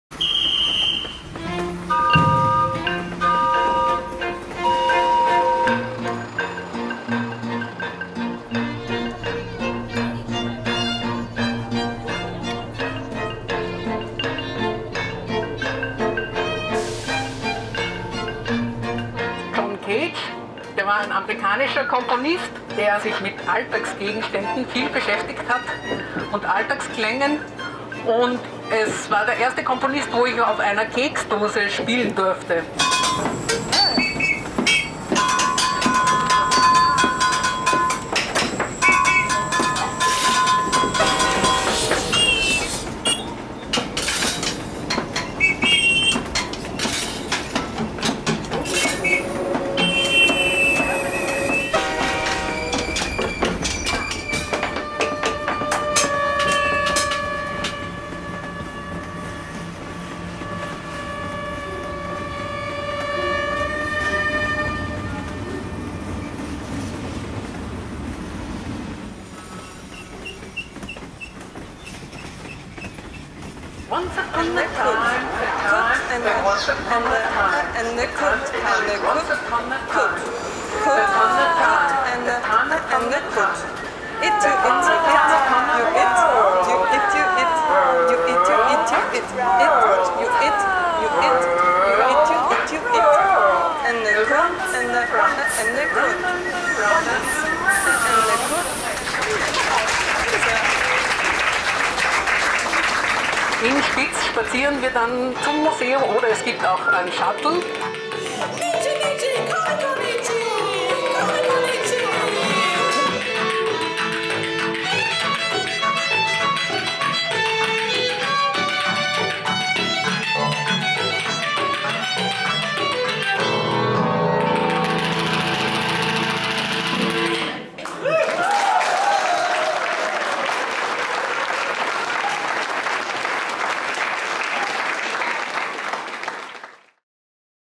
Die Klanginsel «livingTRAINmusic» - ein Videorückblick
Die Klanginsel «livingTRAINmusic» war eine Entdeckungsreise in die Klangwelt von John Cage. Der Komponist gab Alltagsgeräuschen eine Form und modellierte sie zu charakteristischen Musikstücken. Ganz in seinem Stil war die Performance gestaltet, die im April 2018 in der Wachaubahn auf dem Weg von Krems nach Spitz zu erleben war.